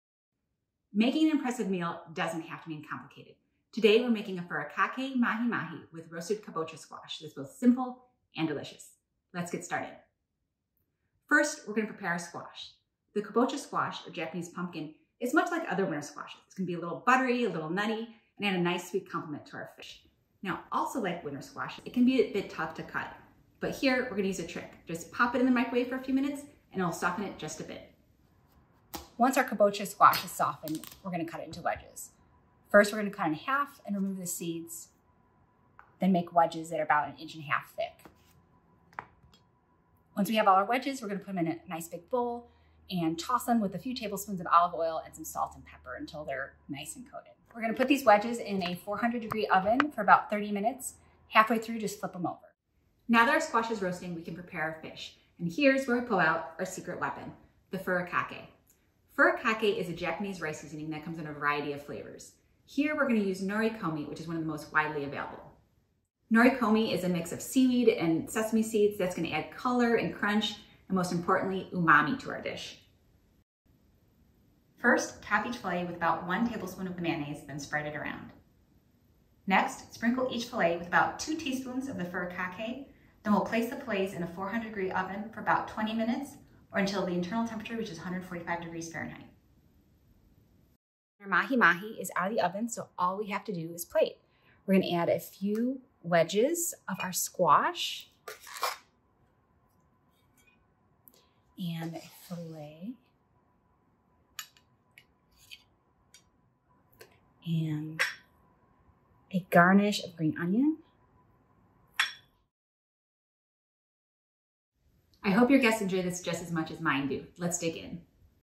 Furikake Mahi Mahi with Roasted Kabocha Squash
This fish recipe is impressive, yet simple and delicious.  My short tutorial demonstrates just how easy it is to make Furikake Mahi Mahi with Roasted Kabocha Squash.